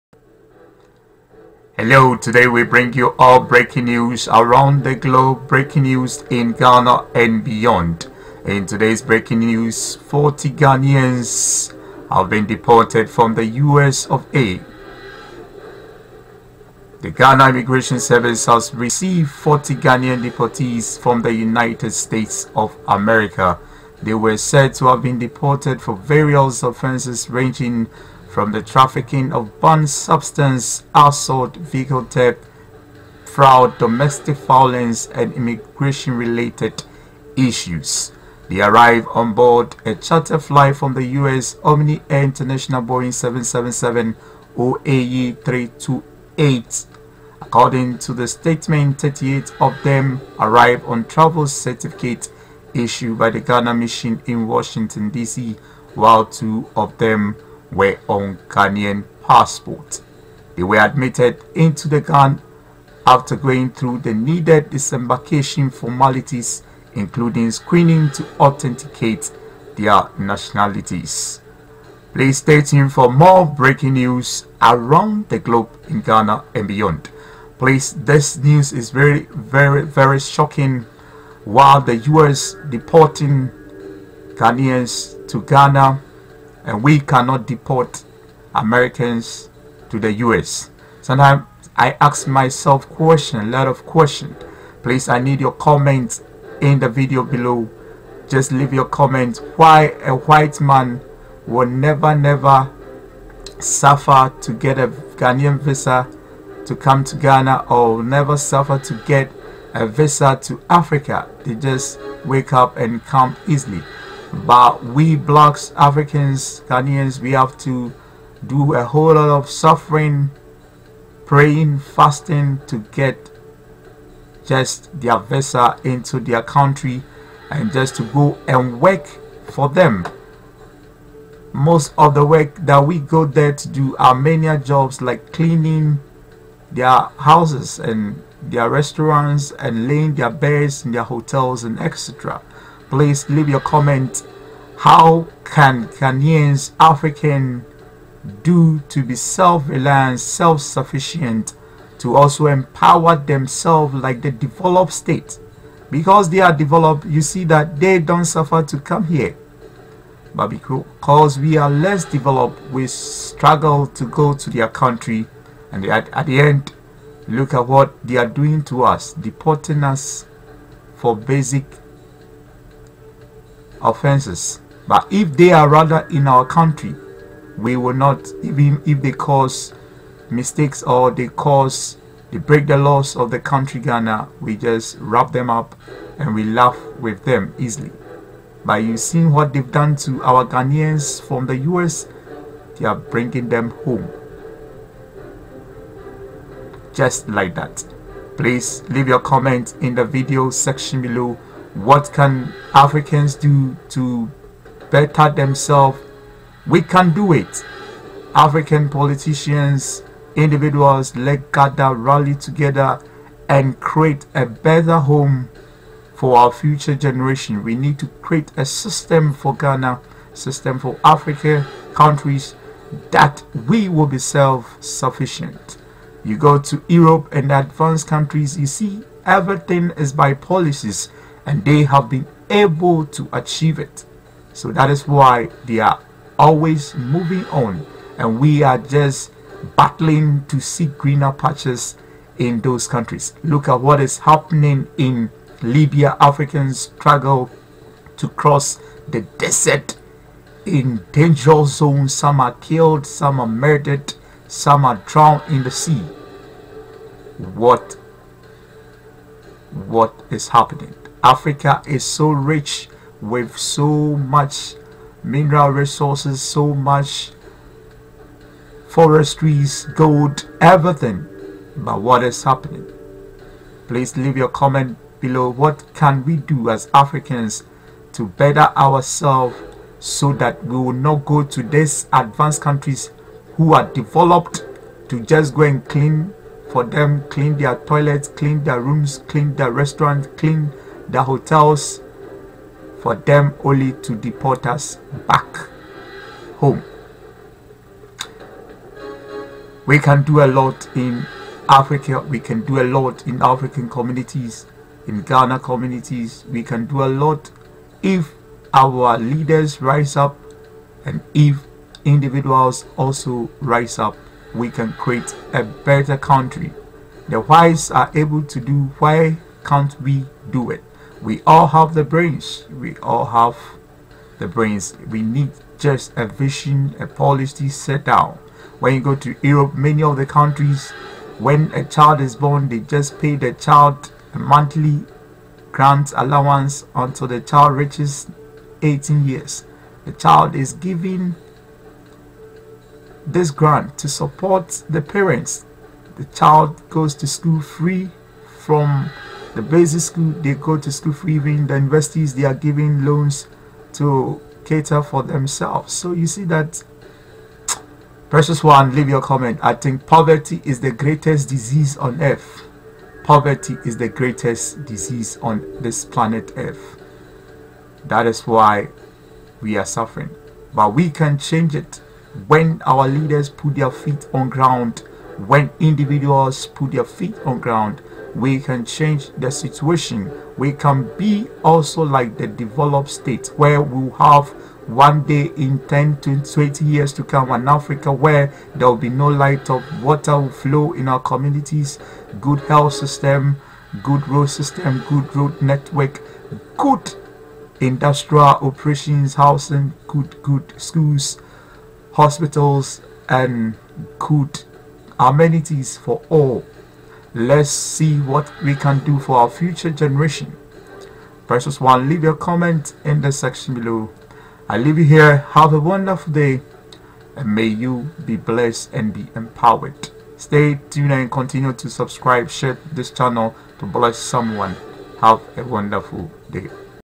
40 Ghanaians Deported from the USA arrive at Kotoka International Airport
40 Ghanaians Deported from the USA arrive at Kotoka International Airport. This video was made by Okai Tv.